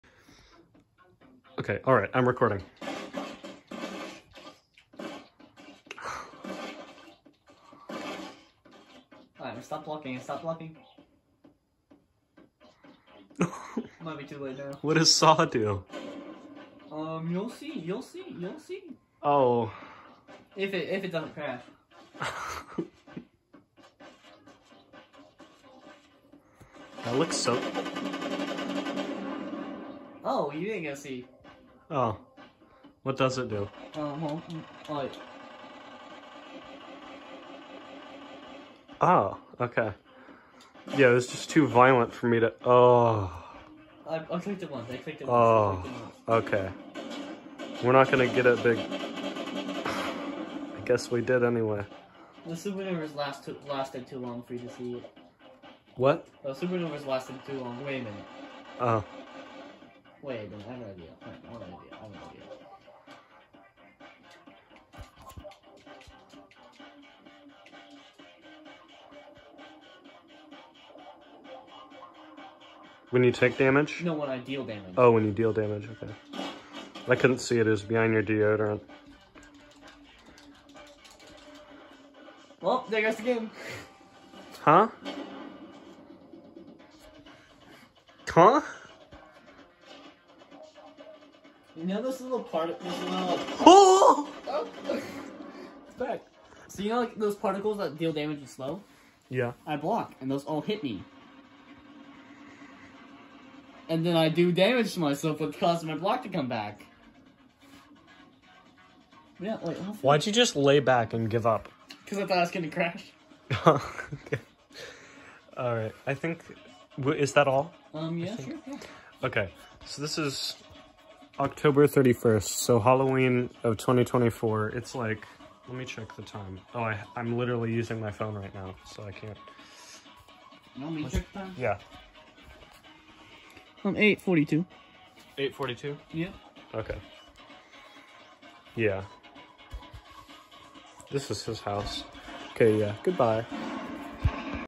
rounds fun
on Halloween night, 2024!! featuring Gannon (me) and Phineas